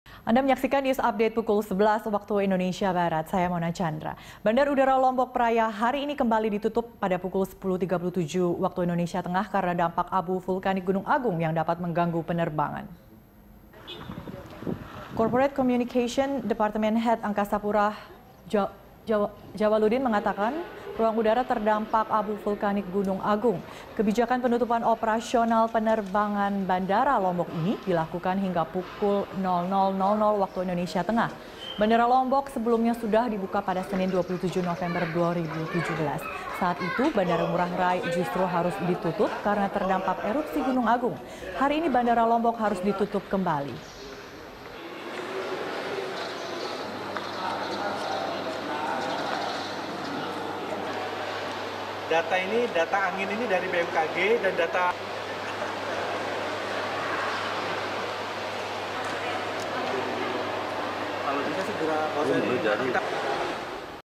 Bandara Lombok Praya Kembali Ditutup Hari Ini
Bandar Udara Lombok Praya hari ini Kamis (30/11/2017) kembali ditutup pada pukul 10.37 WITA. Penutupan dilakukan karena dampak abu vulkanik Gunung Agung yang dapat mengganggu penerbangan.

Facebook.com/BeritaSatuTV
Youtube.com/BeritaSatu